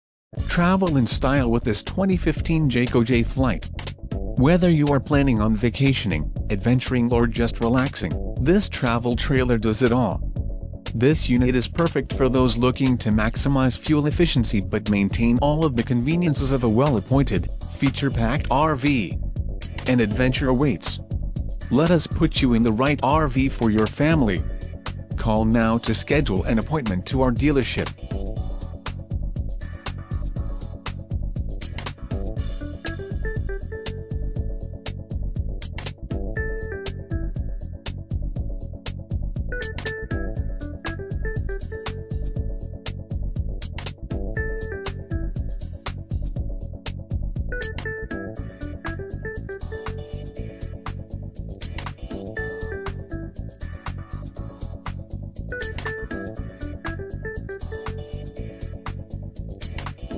2015 Jayco Jay Flight 29QBS 2-Bedroom Sofa/U-Dinette Slid...
America&apos;s bestselling, best warranted travel trailer, since 2005,...and still counting!!Jayco Jay Flight is your answer to a great, quality built travel trailer at a surprisingly affordable price. The 2015 Jayflight has more standard features than others have options. Some features include, I-Class cambered structural steel I-beam frame with fully integrated A-frame, 5/8 tongue-and-groove plywood floor decking completely covered in one piece DiamondFlor vinyl flooring, electric slideouts with enclosed gear system, Radial tires with galvanized-steel, impact-resistant wheel wells, 30x72 main entry door, WaterWorks system with 75 gallon freshwater capacity and durable, winterized Tuff Pex water lines, stabilizer jacks with sand pads (4), Screwed-and-glued realwood cabinetry, Ball bearing drawer guides, 75-lb. capacity and full-drawer extension, Plywood bed, dinette and bunk bases (300-600 ... read more